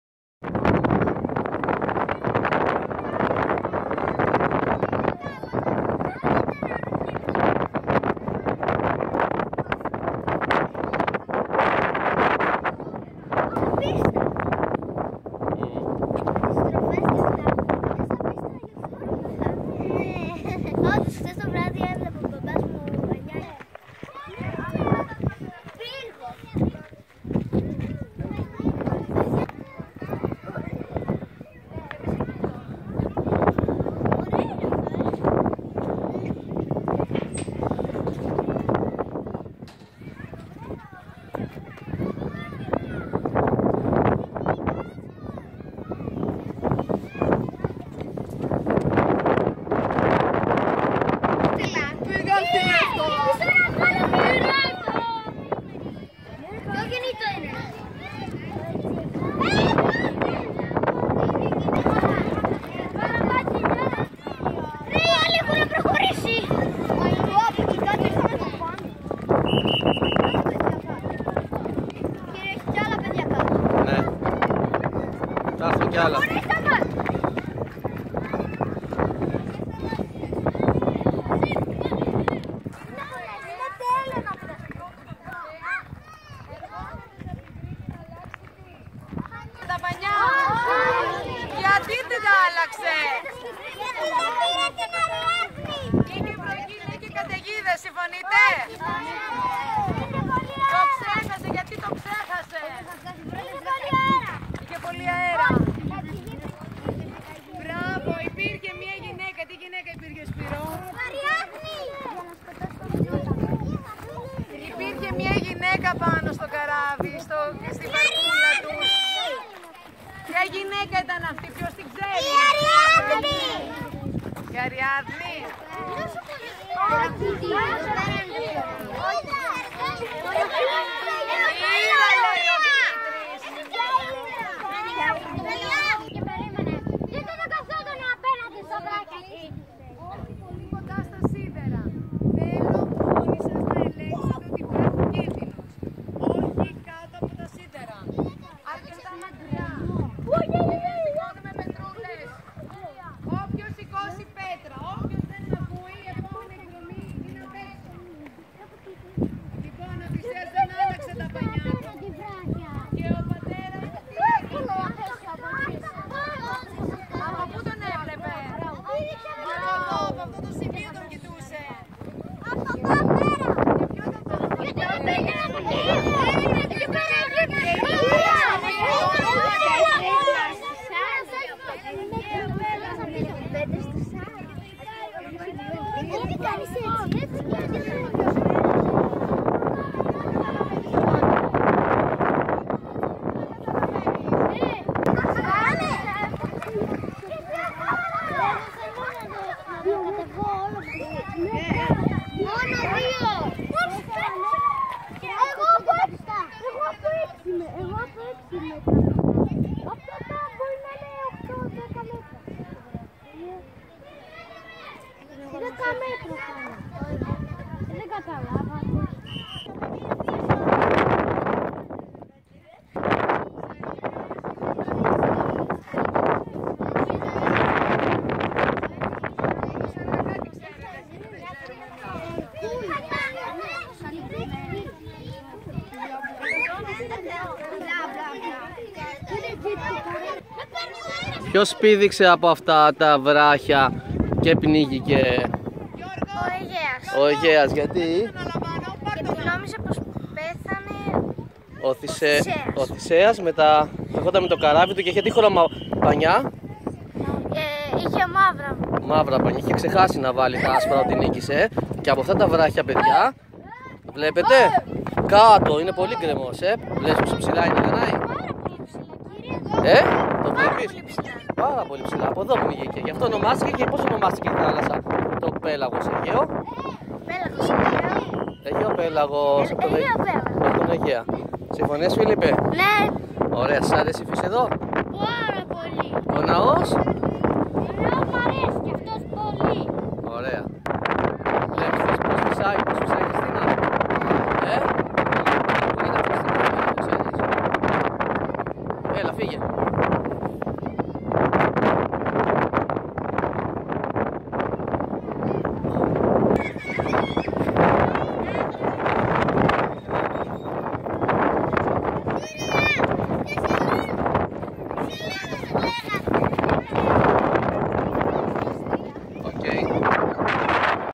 Ο ναός του Ποσειδώνα στο Σούνιο. (5.000 συνδρομητές!!!)
Μαθητική  εκδρομή στο Σούνιο στον ναό του Ποσειδώνα με την τρίτη τάξη του 2ου δημοτικού Αχαρνών 24/10/2014
Επιμέλεια: Γιώργος Δάβος.